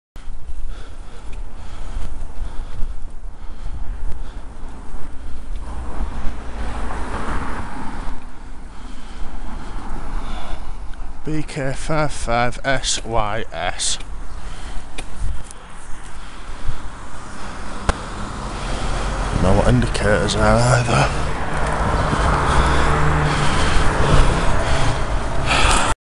BK55SYS - close pass and no indicators
Says it all, had a clear lane to ovetake in, but chose to come within 3ft of hitting me.

Got to the junction and didn't seem to know how to indicate either, which isn't a surprise really